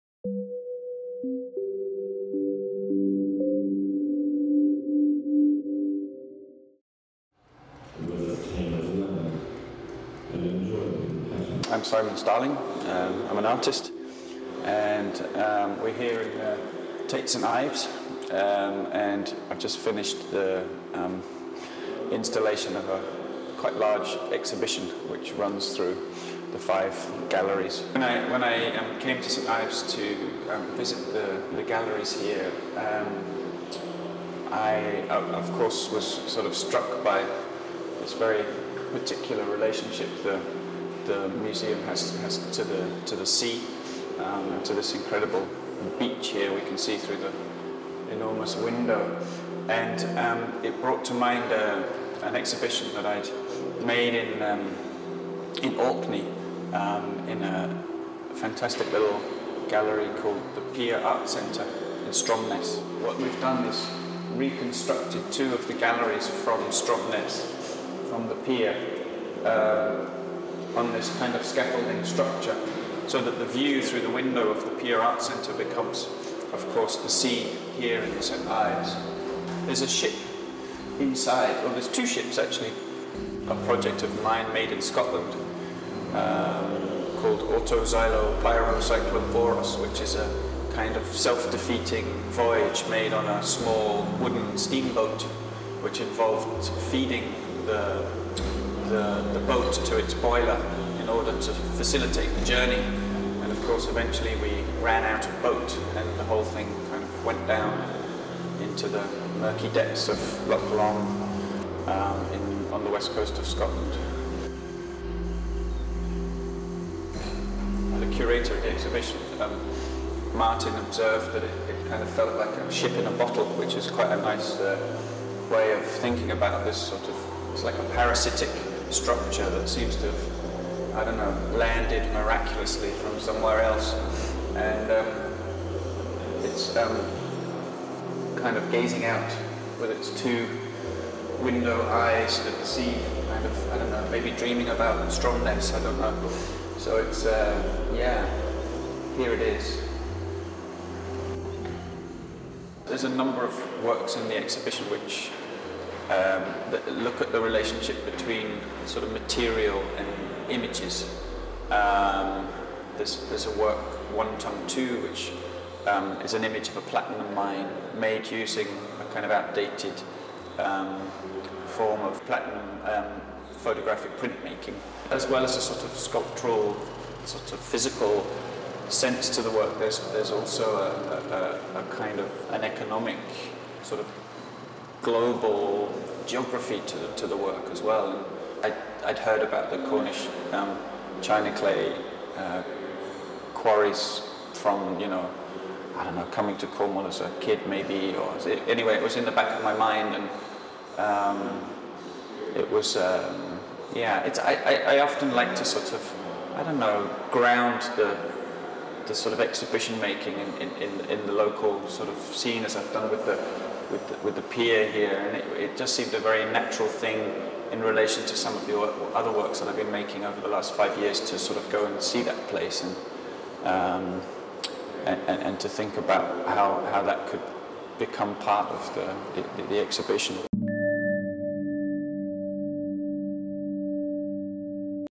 Simon Starling at Tate St Ives | TateShots
Simon Starling  is best known for chopping up a shed, turning it into a boat, sailing it down the Rhine, and then reassembling it as a shed, a work which helped him to win the Turner Prize in 2005.
For his first major exhibition in the UK since, he built an art gallery within an art gallery -- cramming a full-scale replica of the Pier Art Centre gallery in Orkney inside Tate St Ives. We went to Cornwall to talk to him about the show, which draws together works made in the last five years.